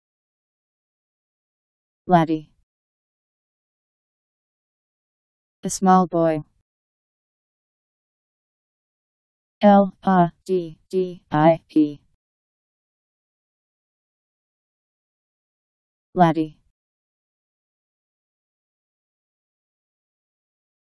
Laddie Meaning
Video shows what laddie means. a small boy.  laddie pronunciation. How to pronounce, definition by Wiktionary dictionary. laddie meaning. Powered by MaryTTS